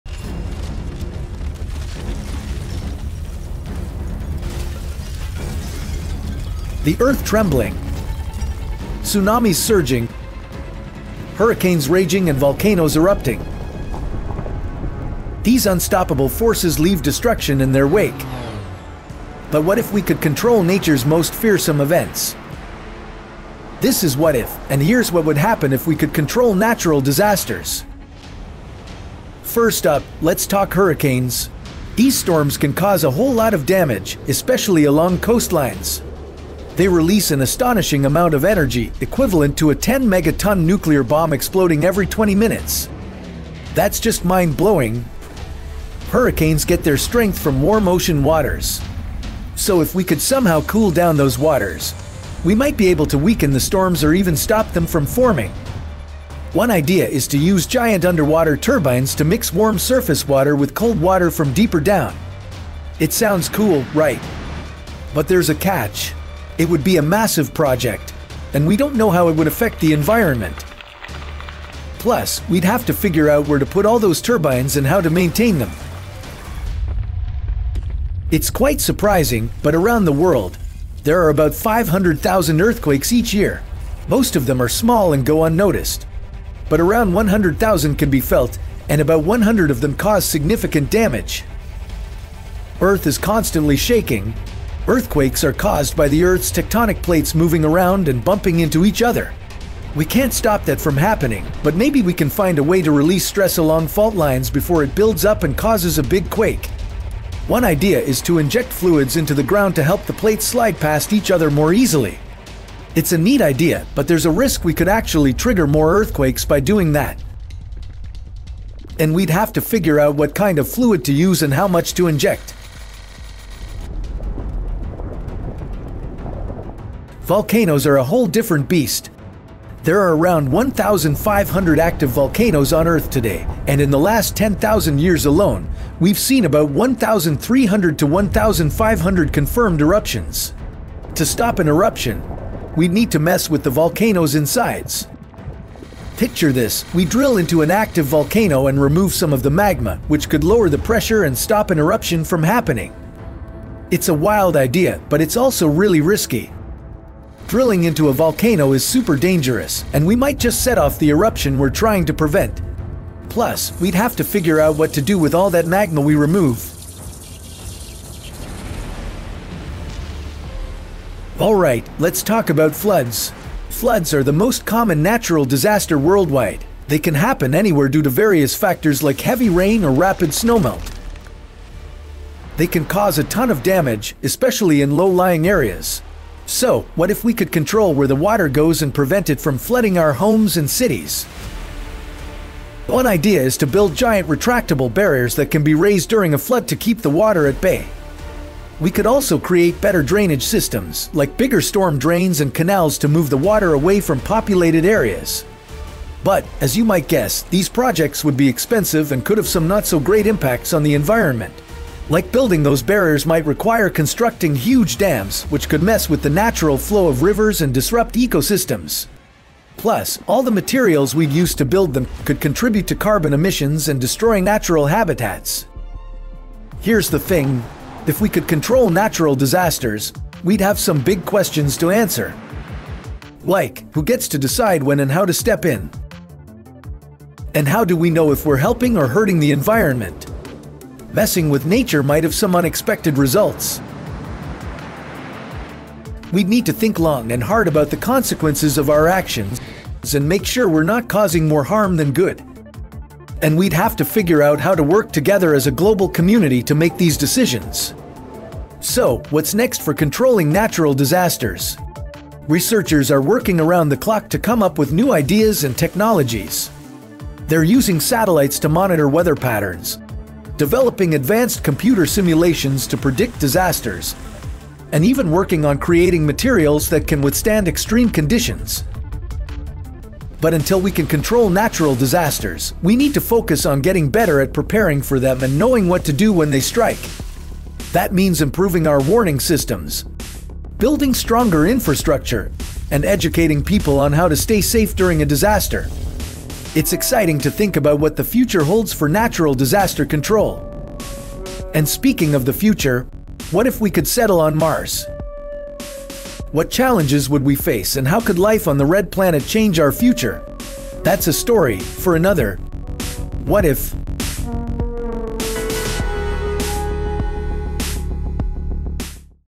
如果我們能控制自然災害【中文字幕】｜大膽科學
大地晃動，海嘯湧蕩，颶風肆虐，火山噴發。這些勢不可擋的力量於所到之處必破壞萬物。可是呢，如果我們能控制自然界最可怕災害會怎樣呢？